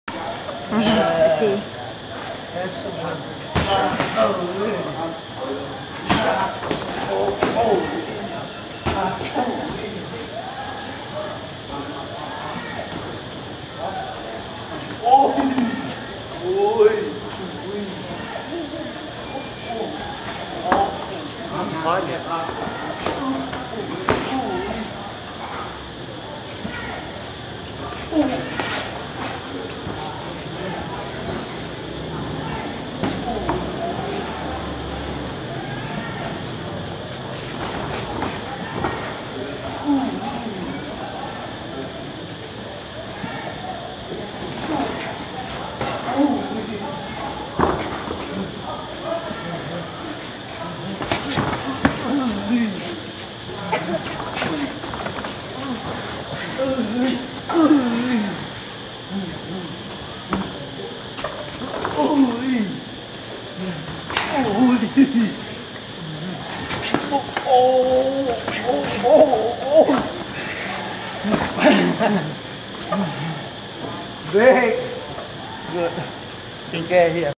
Учебный спарринг. Михаил Шилов и Сават
Муай тай. Тайгер кэмп. Пхукет. Тайланд. Весна 2013 г.